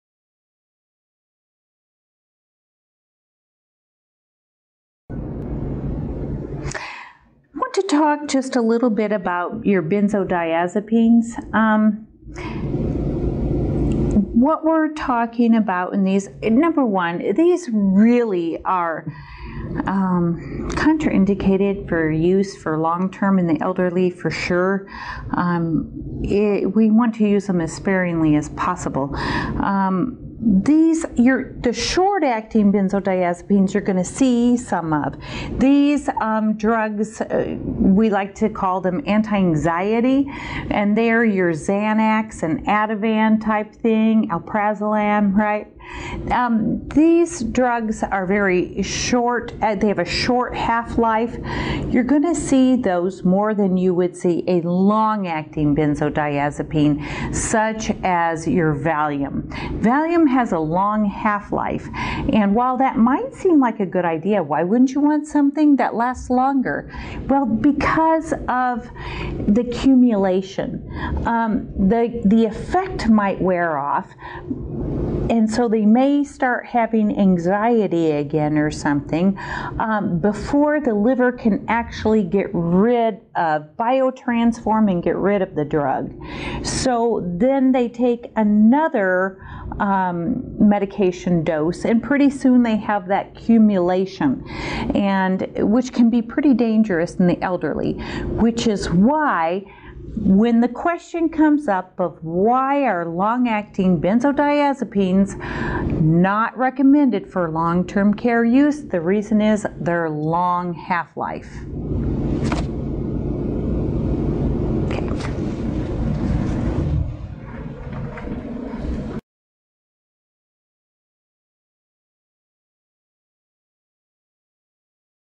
CMA Benzodiazapenes
Dianne Powell, owner of Allied Health Career Training, discusses benzodiazapenes as part of the certified medication aide course offered by Allied Health Career Training.